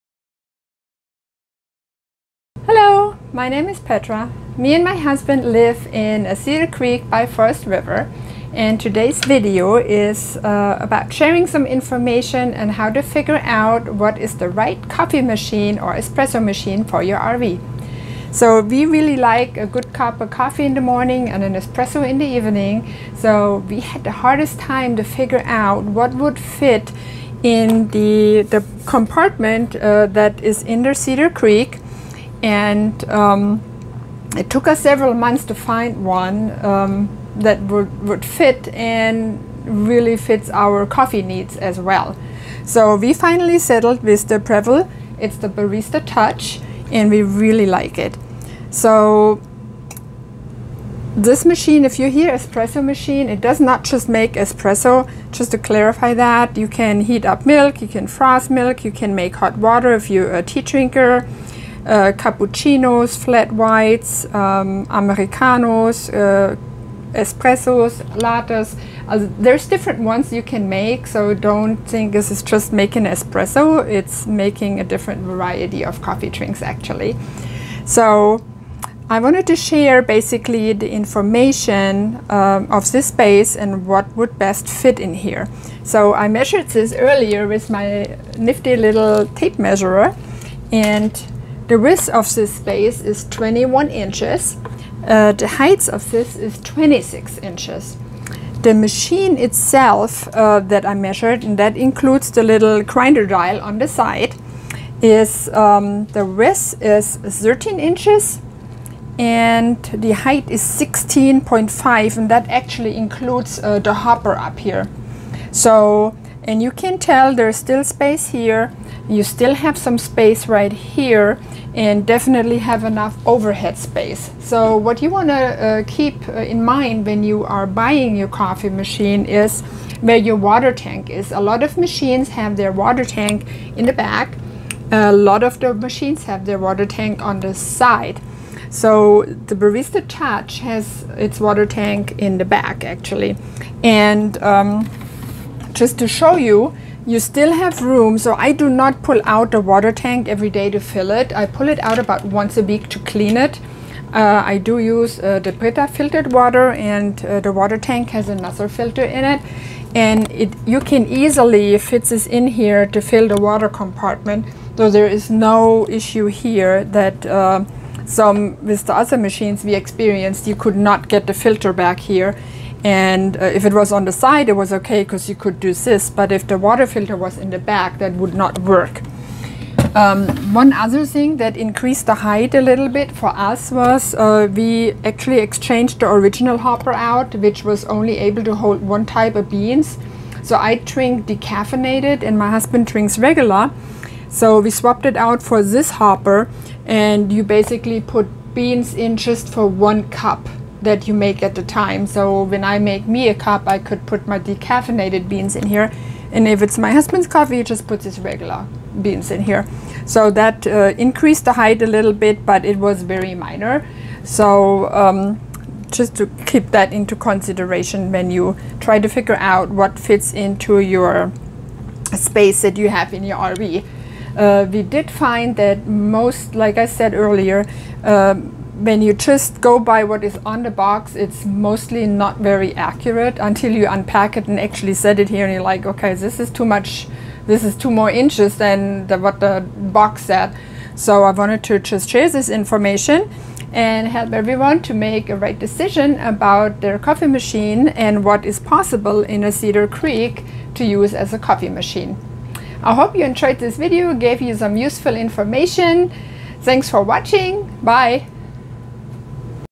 Best Fitting Coffee Machine RV and Campers
Are you having trouble to find the perfect fitting coffee machine for your RV?  In my video I am showing you the perfect fitting Coffee machine for our 5th Wheel the Cedar Creek by Forest River. We have a 571FL 2021 and loving it.

Are you camping a lot, but also love a good coffee, cappuccino, or just need hot water for tea? So do we, we found the perfect coffee machine with the Breville Barista Touch.

Turn you dedicated space into a coffee bar and become your own Barista. Safe that money you normally spend at Starbucks.